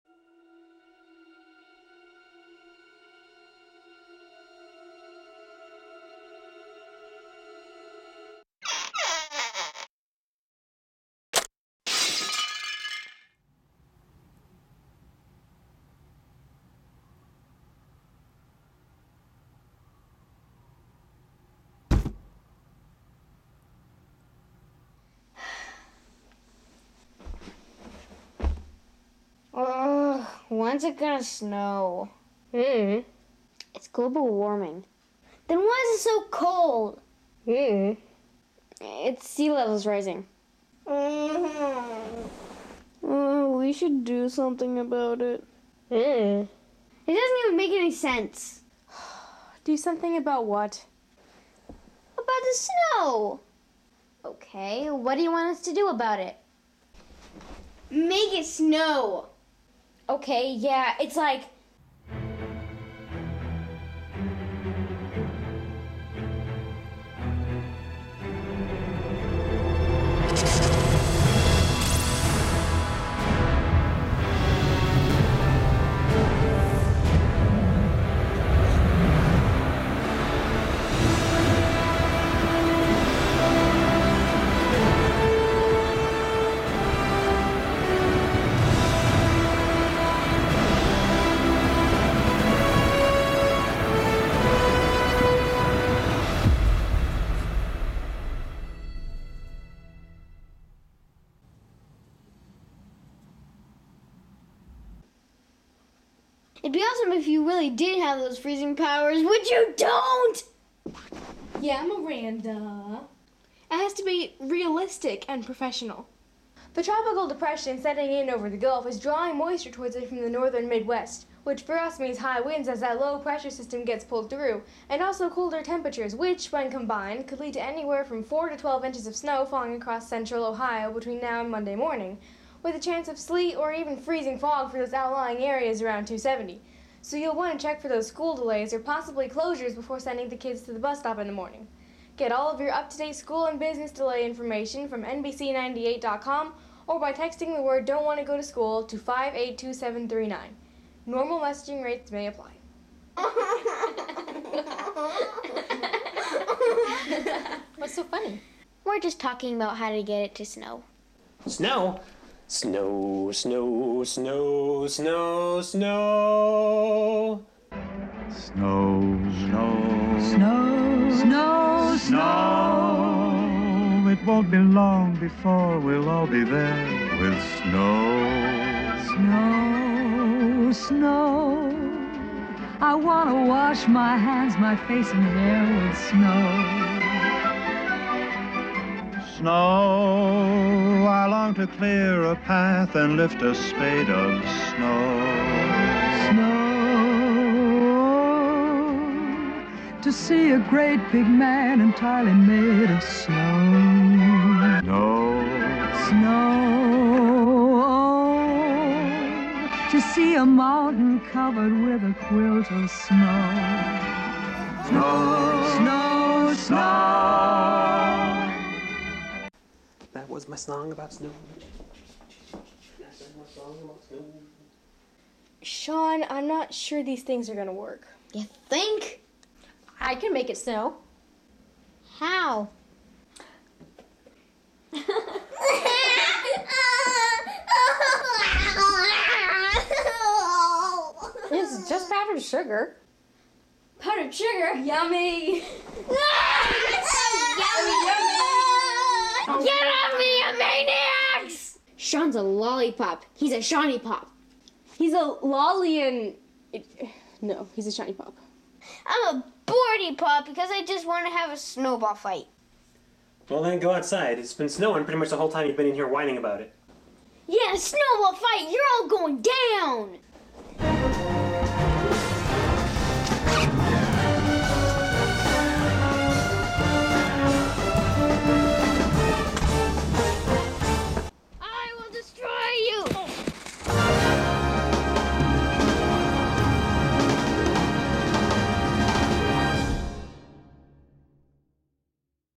Snow
Such a dry winter--Sean just wants it to snow so he can demonstrate his snowball fighting supremacy.